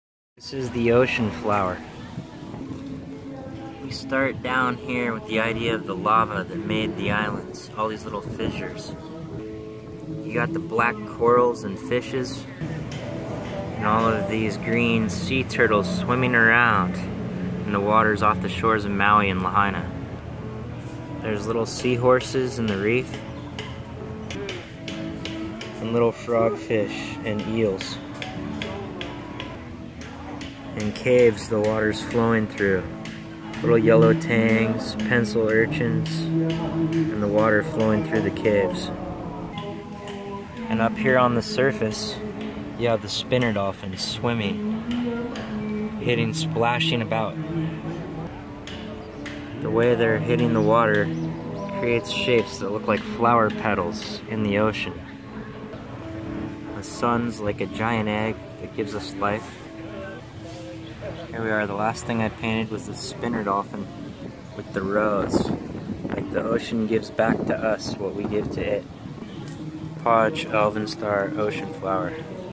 Hawaii Artist, Ocean Flower, Podge Elvenstar, Maui oil Painting
Dolphin Explossion, Maui Painting showing the heart of the ocean.   All of the abundant life and inspiration from swimming in the Maui reefs.